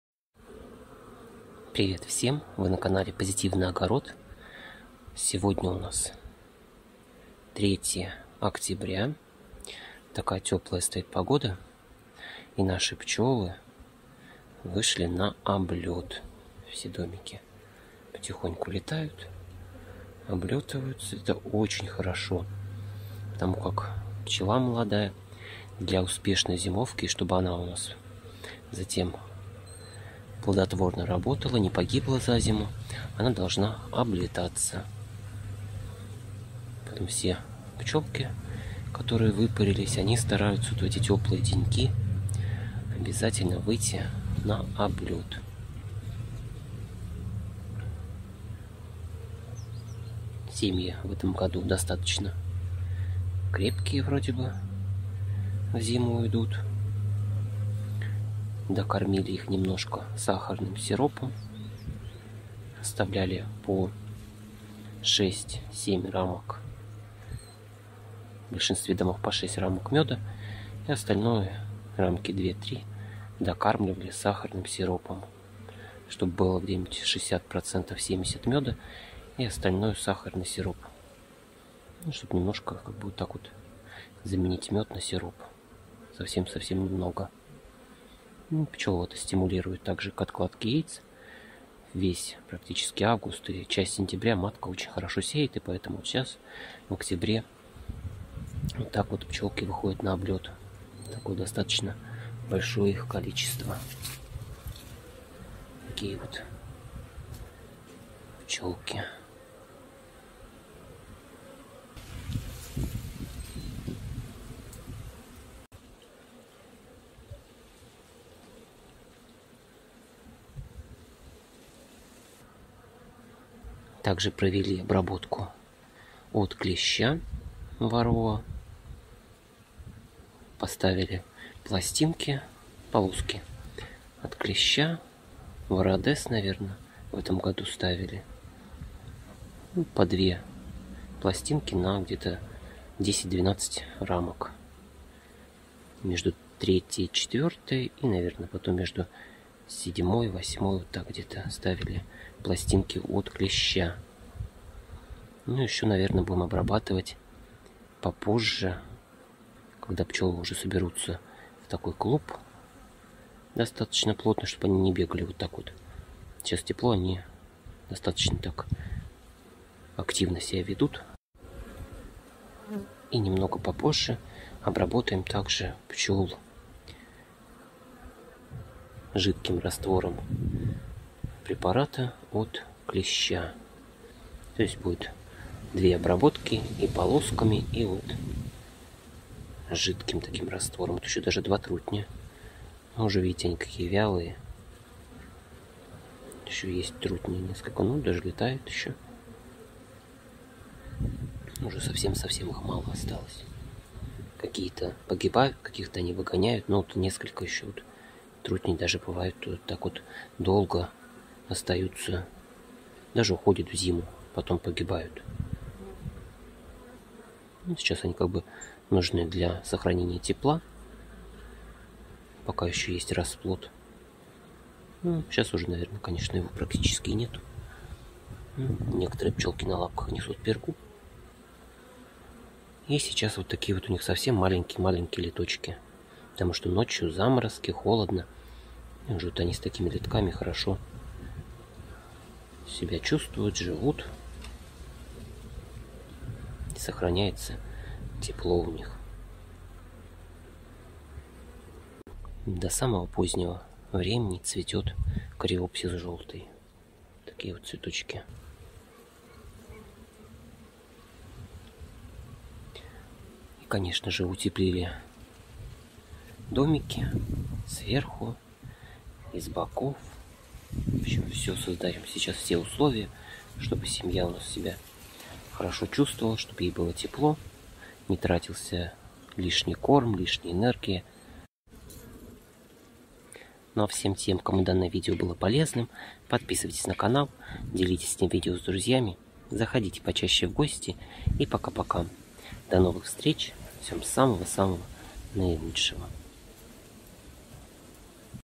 Последний облет пчел перед долгой зимовкой
3 октября 2020 года.
Очень важно, чтобы пчелы провели очистительный облет перед продолжительной зимовкой. Сделать это им желательнее, как можно позднее, но в Сибири это не всегда удается сделать по причине погодных условий. Но чаще всего в октябре случается несколько теплых деньков, когда пчелы и выходят на облет.
Все пчелиные семьи были докормлены сахарным сиропом и обработаны от клеща варроа. Провели утепления гнезд и сверху (ватные положки) и с боков (пенопласт).